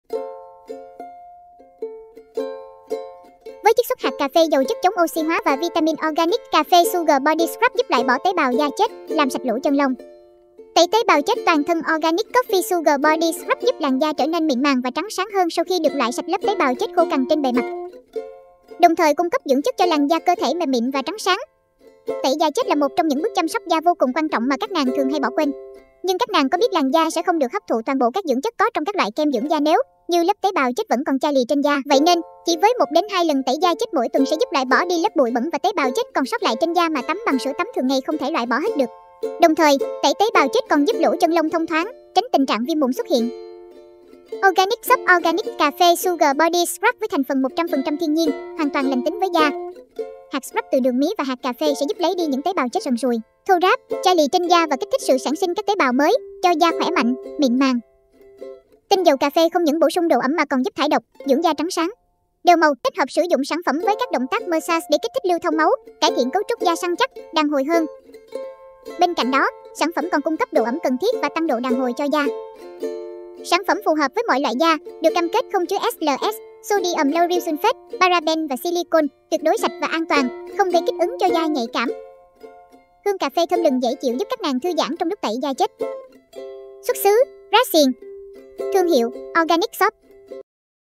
Tẩy tế bào chết toàn thân Organic Coffe Sugar Body Scrub 250ml
@myphamchinhhanglongan Với chiết xuất hạt cà phê giàu chất chống oxy hóa và vitamin organic coffee sugar Body Scrub giúp loại bỏ tế bào da chết, làm sạch lỗ chân lông

Tẩy tế bào chết toàn thân Organic Coffe Sugar Body Scrub giúp làn da trở nên mịn màng và trắng sáng hơn sau khi được loại sạch lớp tế bào chết khô cằn trên bề mặt.